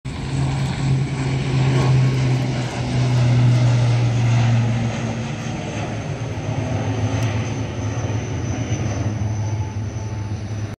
ALLIANCE AIR ATR72-600 TURBOPROP TAKEOFF
EVENING TAKEOFF OF AN ALLIANCE AIR ATR72-600 TURBOPROP (VT-RKL/91894) FROM RUNWAY 07 OF CHENNAI AIRPORT TO HYDERABAD